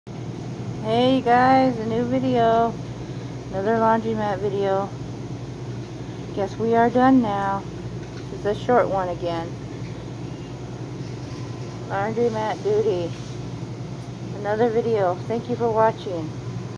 The Laundrymat video
A very short video of me and the dryer had stopped but that is my clean laundry!!😉😁👍